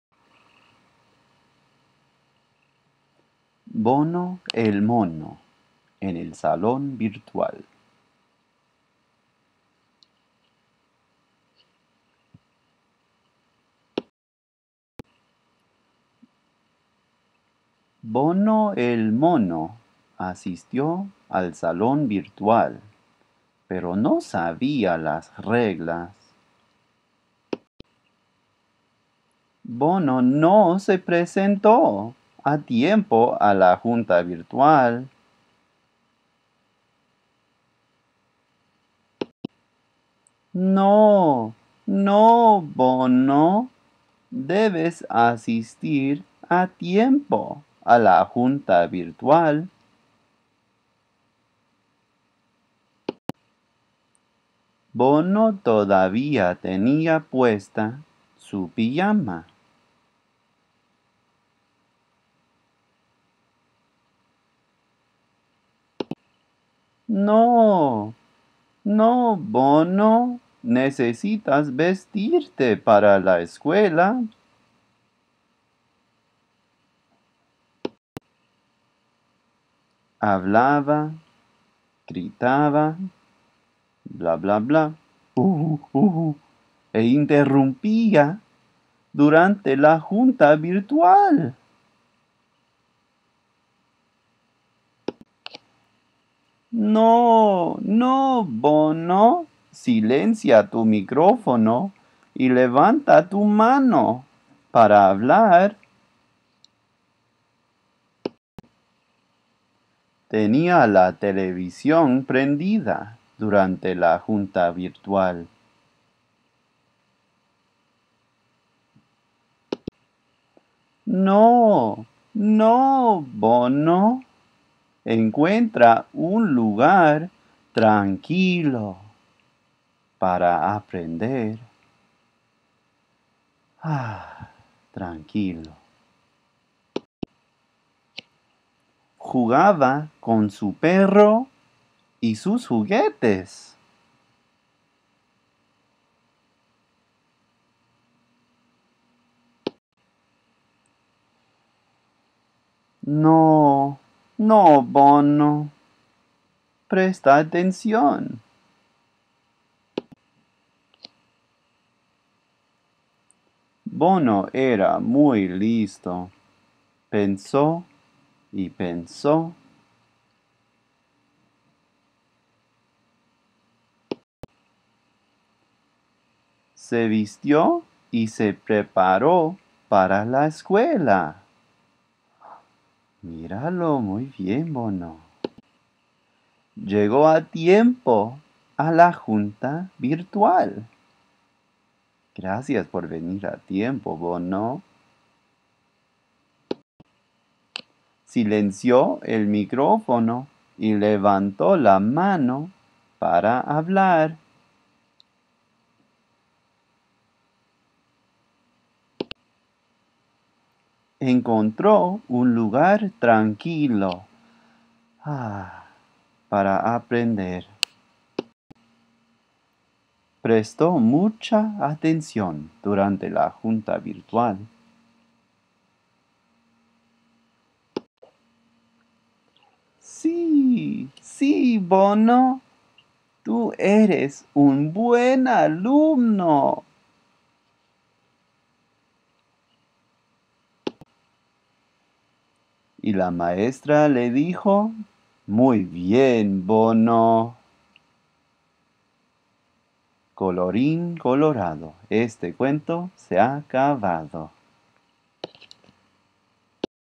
Bono El Mono Para escuchar
¿Qué son las normas en el salón virtual?  Bono aprende en este libro como se porta un buen alumno en el salón virtual.